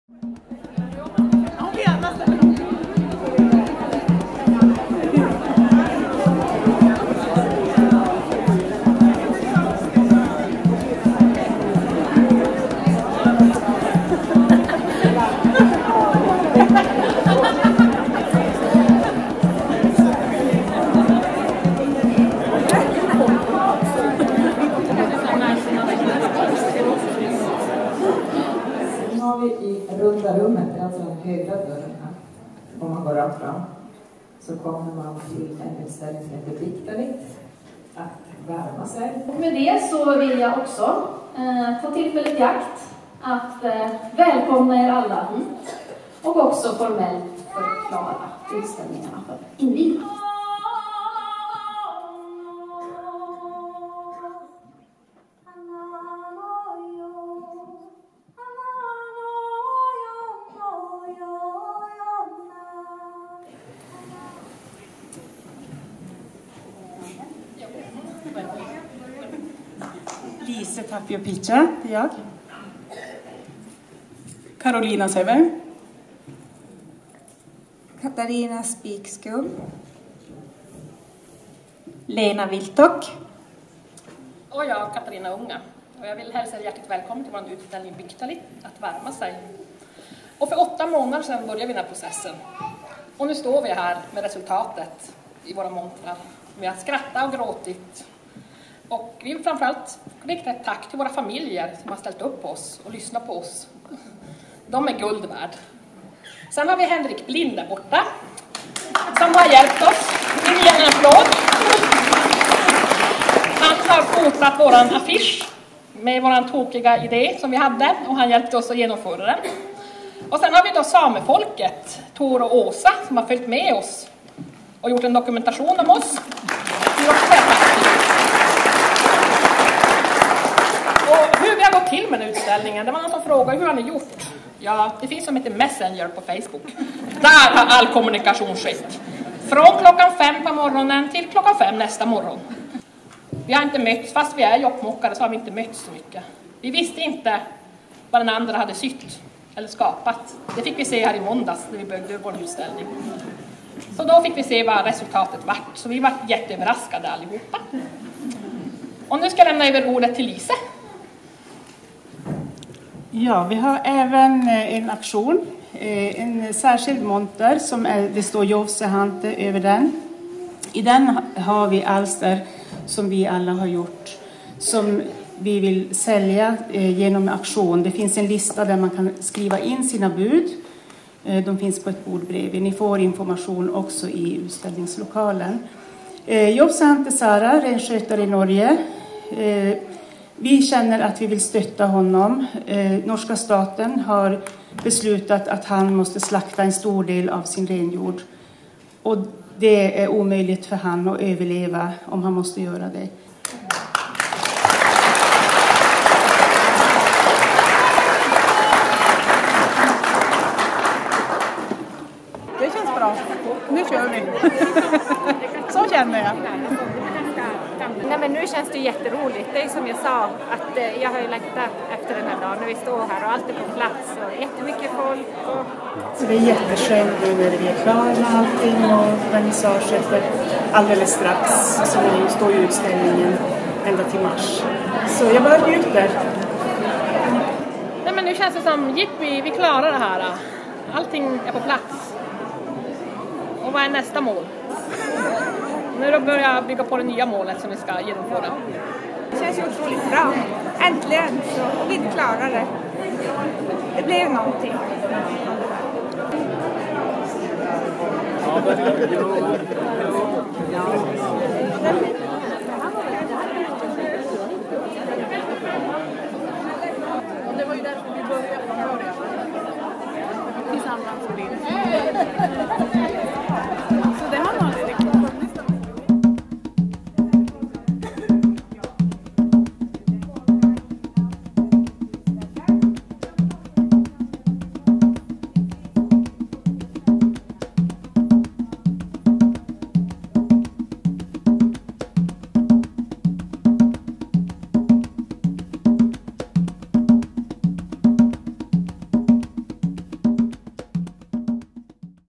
Biktalit, vägen mot vernissage 8
Fem slöjdare. En utställning. Dagen för vernissage har kommit. Det är fullt med folk i Gamla hallen på Ájtte när Biktalit och fyra andra utställningar öppnas av nya kultur-, demokrati- och sameminister Amanda Lind. Se Samefolkets sista del om fem mjukslöjdares väg mot sitt mål. Alla videor finns samlade på